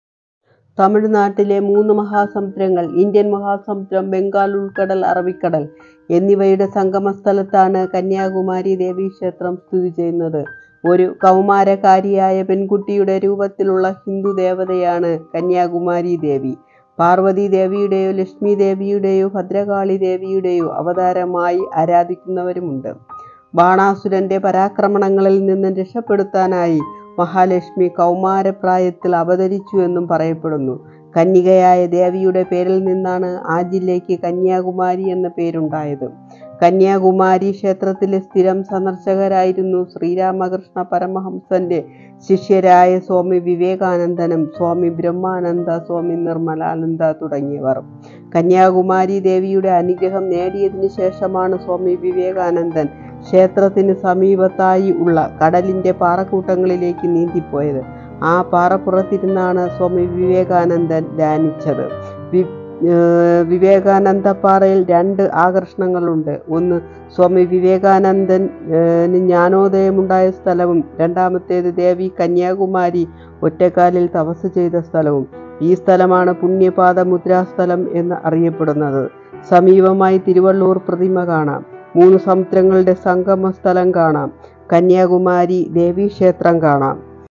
കന്യാകുമാരി ഭഗവതി അമ്മൻ ക്ഷേത്രം video hindutemple 2024 01 02 23 09 11@sssi6872
nagarkovil / ഇന്ത്യയുടെ തെക്കേ അറ്റത്തുള്ള ജില്ലയാണ് കന്യാകുമാരി/ മൂന്നു മഹാസംഗങ്ങൾ സംഗമിക്കുന്ന സ്ഥലത്താണ് കന്യാകുമാരി ദേവി ക്ഷേത്രം സ്ഥിതി ചെയ്യുന്നത് /
Voice --Indulekha
YTCREATE